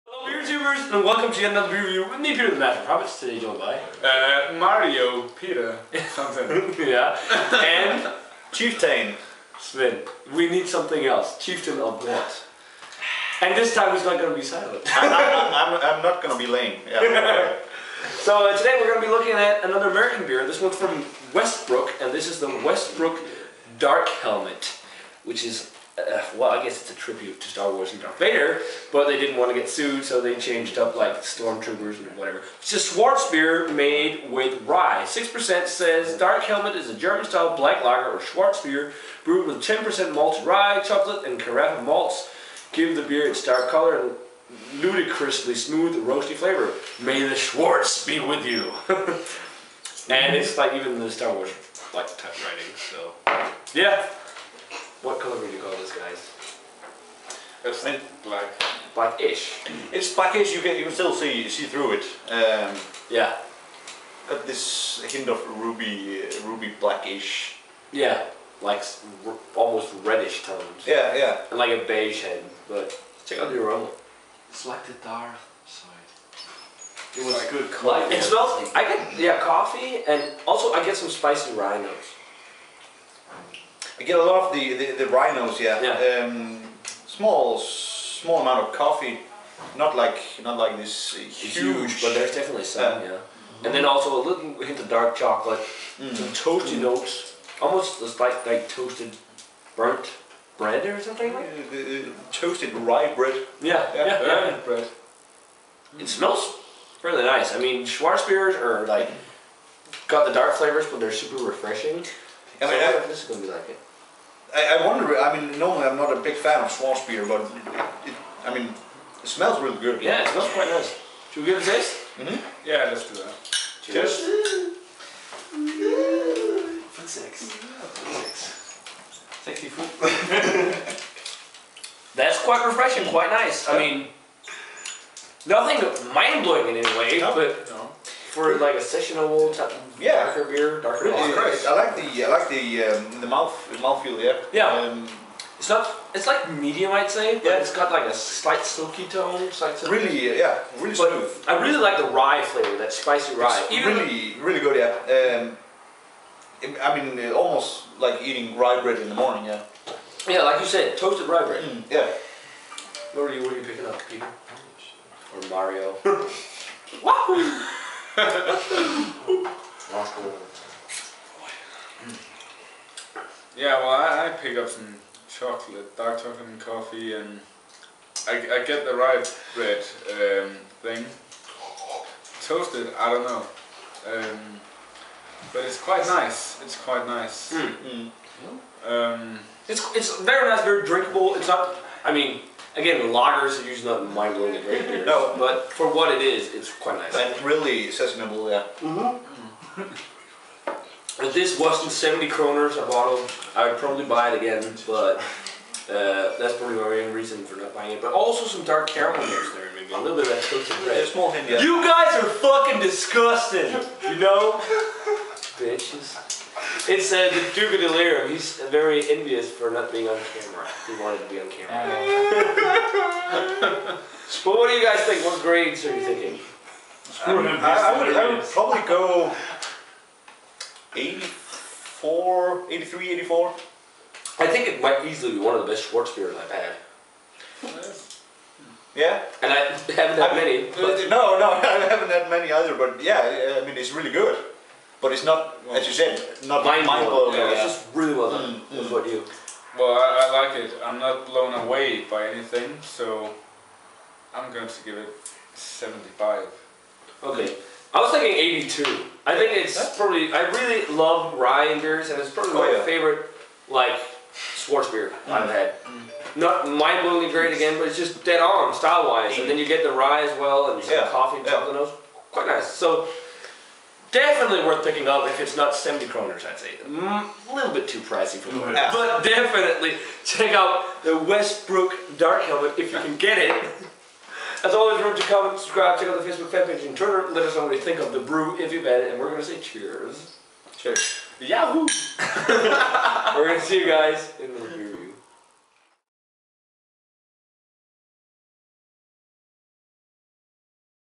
TMOH - Beer Review 1298#: Westbrook Dark Helmet
Information About This Beer:

Brewery: Westbrook Brewing
Beer: Westbrook Dark Helmet
Style: Schwarzbier
Location: Brewed by Evil Westbrook Brewing in Mt. Pleasant, South Carolina, USA.
ABV: 5,9%
Rating: 82/100